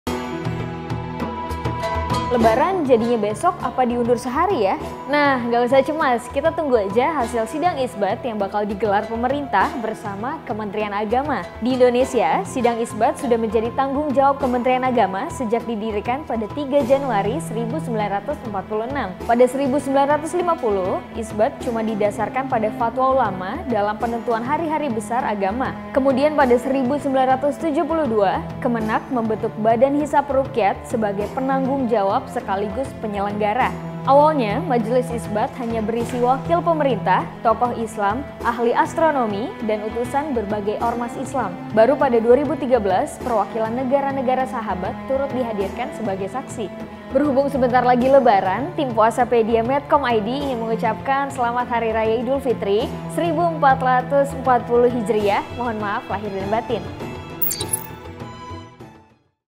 PuasaPedia - Sidang Isbat
Sidang Isbat menjadi penentu Hari Raya Idulfitri di Indonesia sejak lama. Namun, bagaimana sih sejarahnya? Yuk, simak penjelasannya di PuasaPediaMedcomID!

Konten spesial dari Medcom.id akan menemanimu jelang buka puasa selama Ramadan2019, lho!